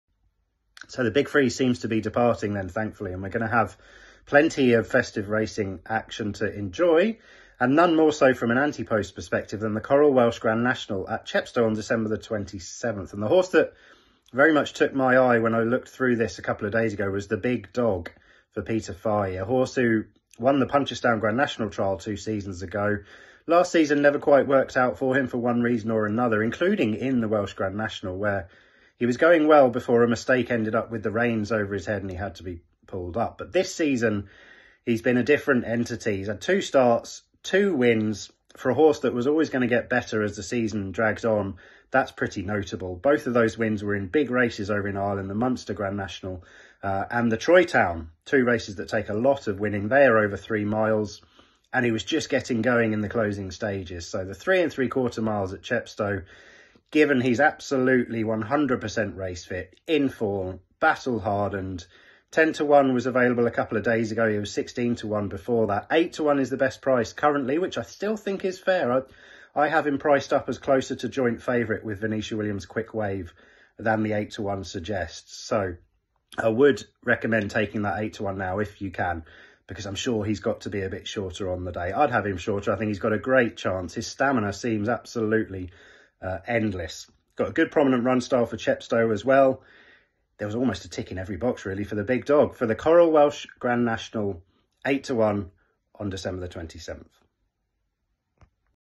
Coral Welsh Grand National Preview & Betting Tips - Will Smith
Horse racing betting tips for the Coral Welsh Grand National at Chepstow on Tuesday 27th December. Horse racing betting tips from racing analyst Will Smith include a 10/1 ante-post each-way selection.